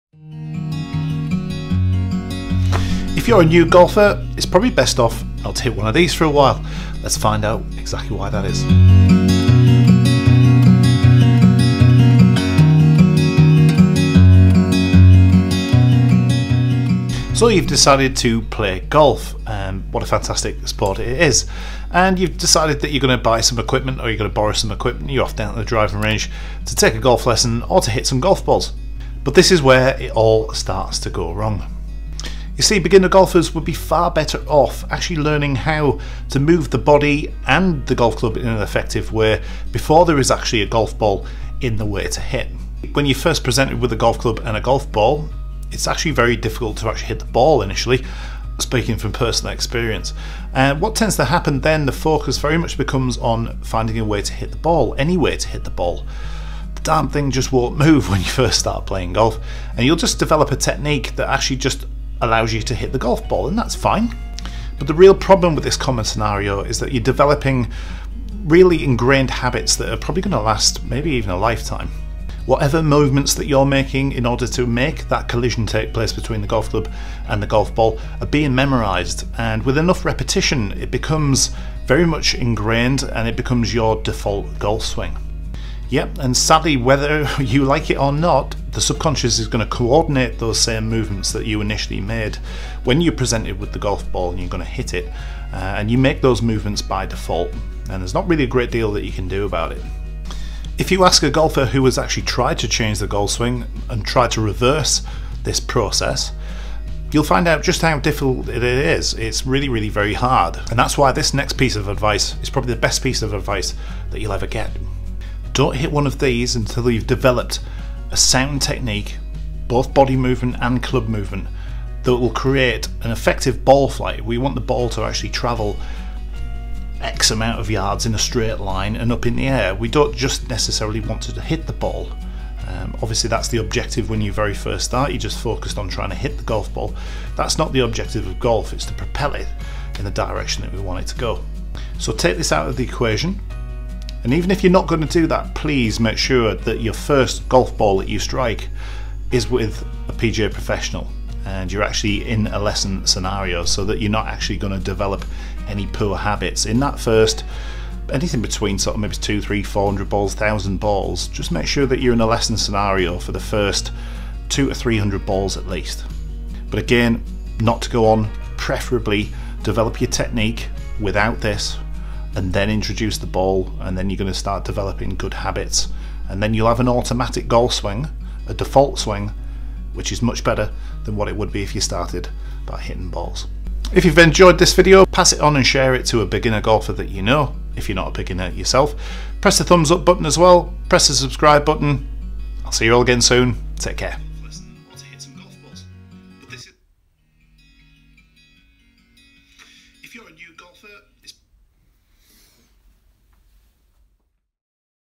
GOLF ADVICE FOR BEGINNERS
In this video I discuss why I believe it is crucial that before a beginner golfer hits balls his/her technique needs to be functional first!